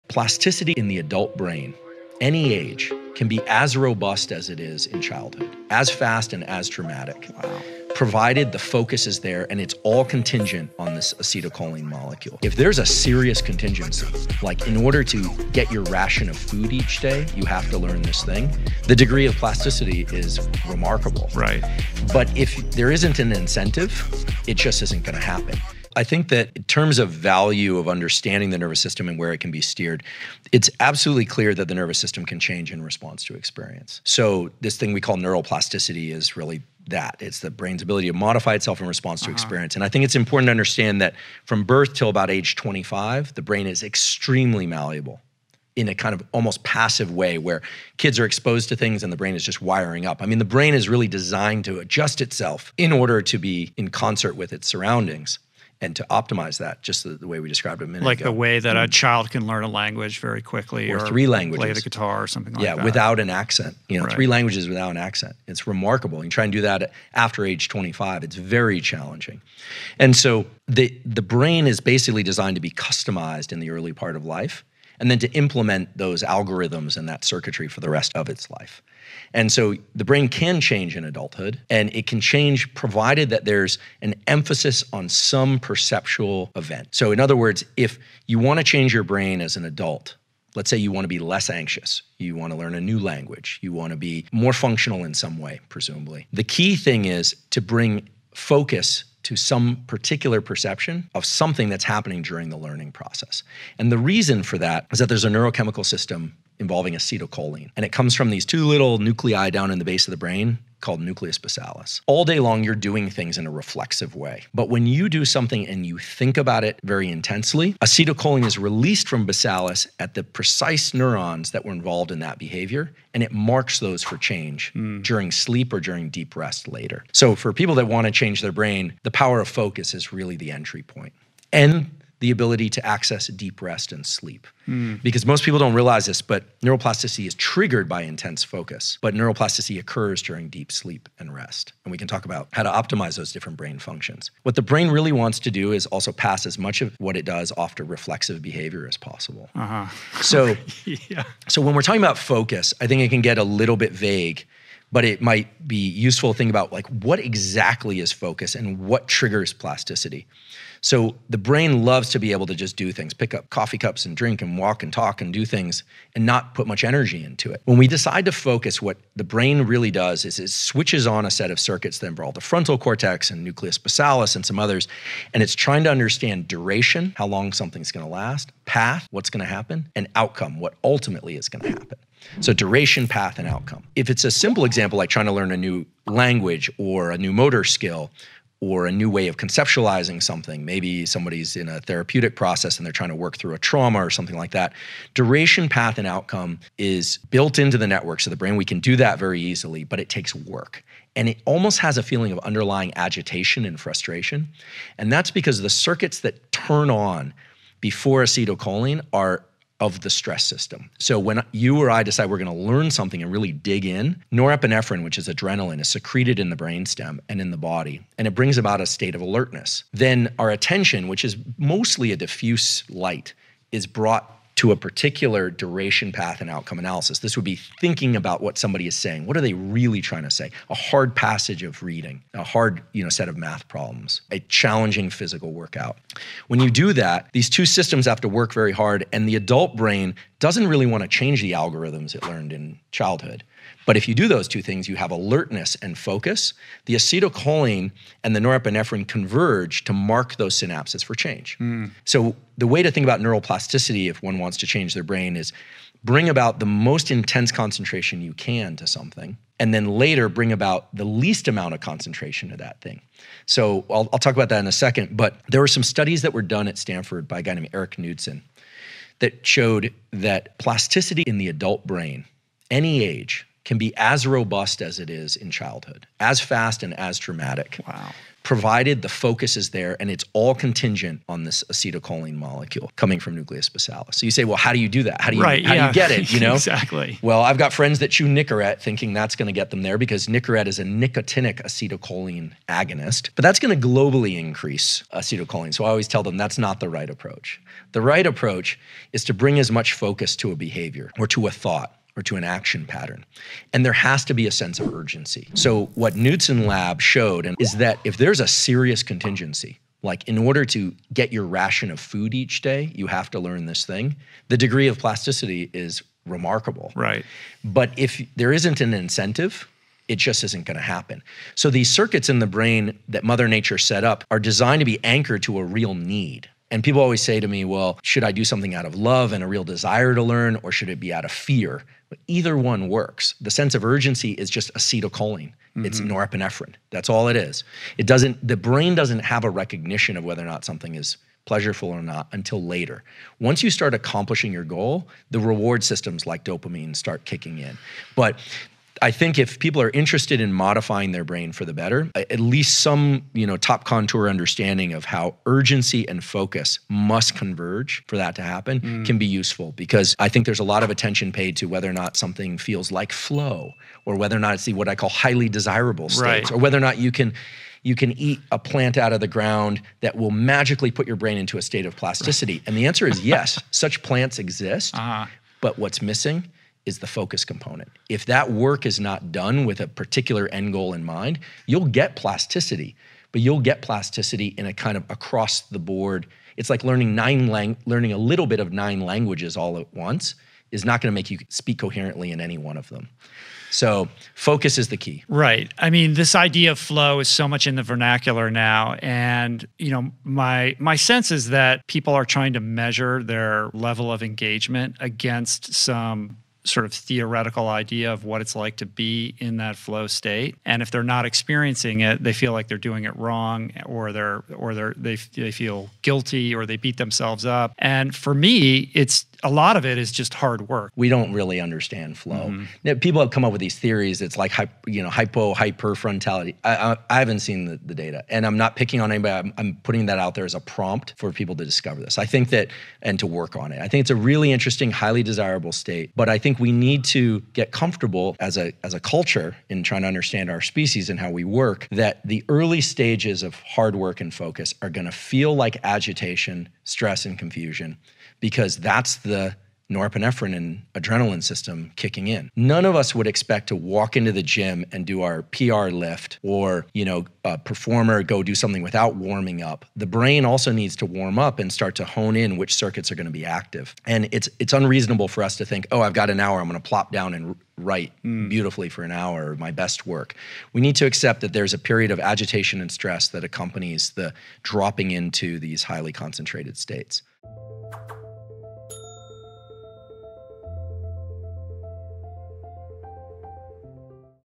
UNDERSTAND Your BRAIN and LEARN ANYTHING | Andrew Huberman
Professor Andrew Huberman, PhD explains the precise brain wiring that fires up when focusing and studying, offering simple tricks to make your mind a flawless calculator machine.